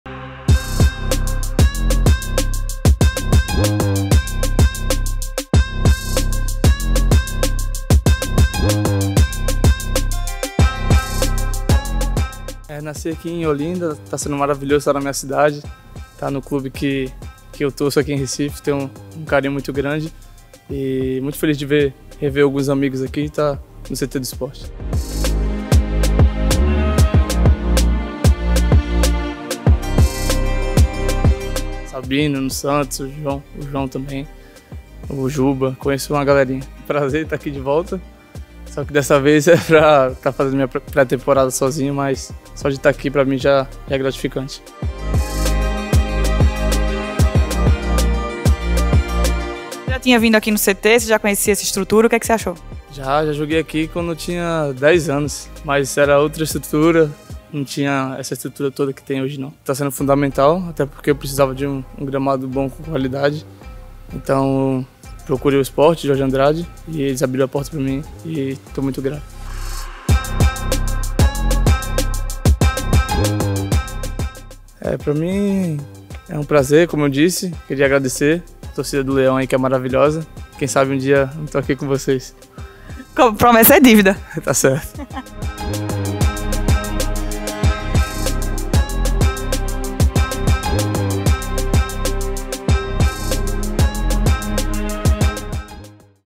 KAIO JORGE TREINA NO CT RUBRO-NEGRO
CT de excelência! 🔥 Recebemos mais um atleta realizando seus treinamentos no Brasil: Kaio Jorge esteve por aqui e trabalhou muito!

Imagens: Júnior Nuno e Igor Cysneiros